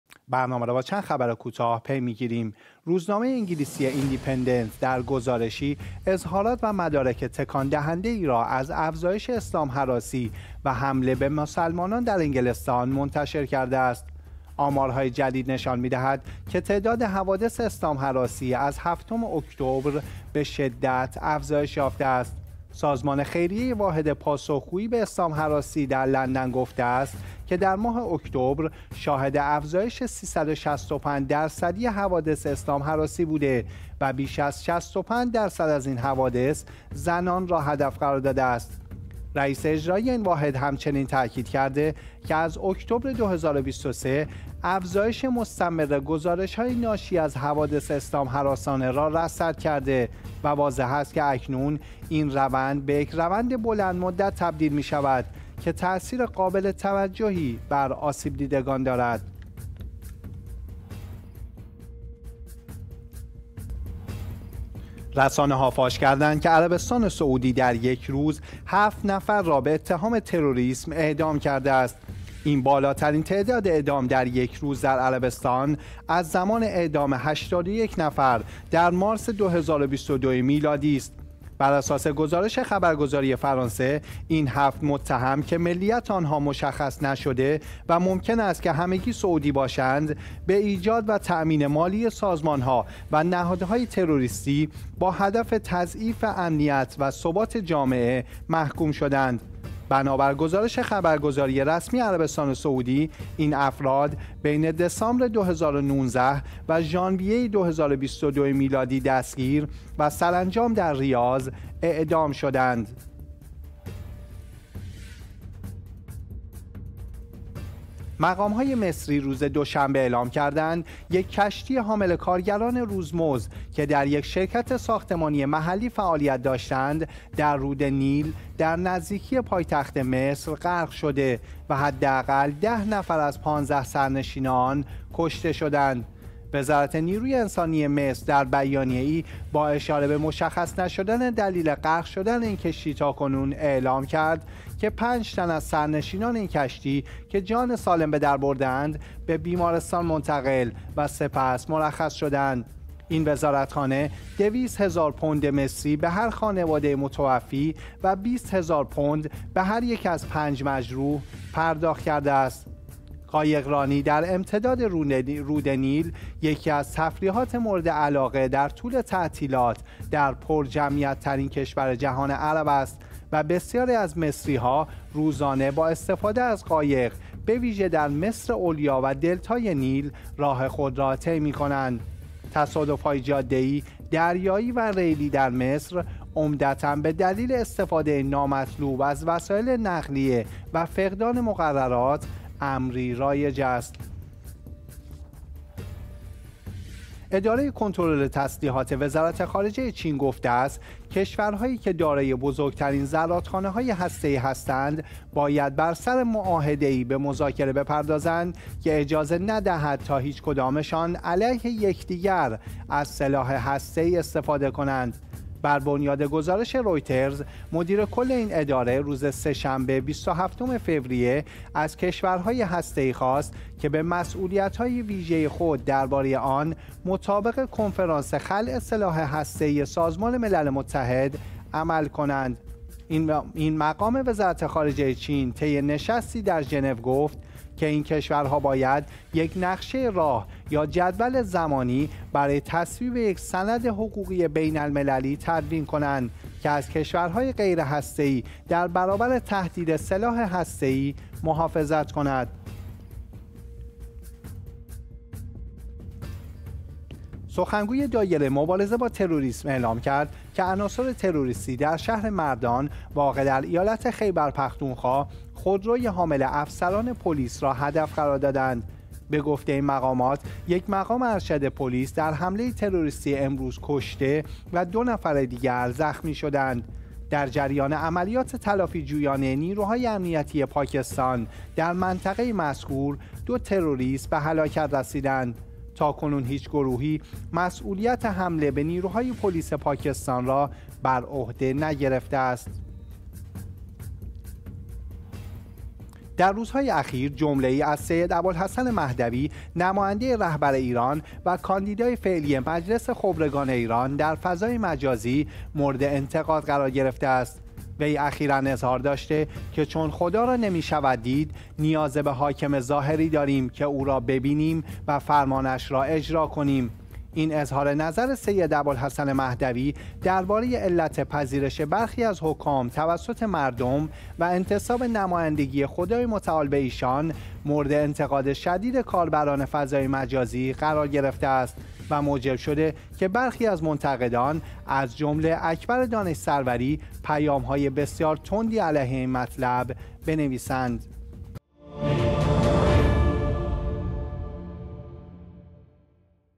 نگاهی به چند خبر کوتاه: چهارشنبه 9 اسفند ماه 1402
1) افزایش اسلام هراسی و اهانت به مسلمانان در انگلیس
2)اعدام ۷ نفر به اتهام تروریسم در عربستان سعودی
3) کشته شدن دست کم ۱۰ نفر بر اثر غرق شدن کشتی در رود نیل مصر
4) چین خواستار مذاکره درباره معاهده عدم استفاده ‌از سلاح اتمی شد
5) حمله تروریستی به پلیس پاکستان با یک کشته و سه زخمی
6) اظهارات جديد نماينده رهبر ايران كه موجب هياهوى جديدى در فضاى مجازى شده است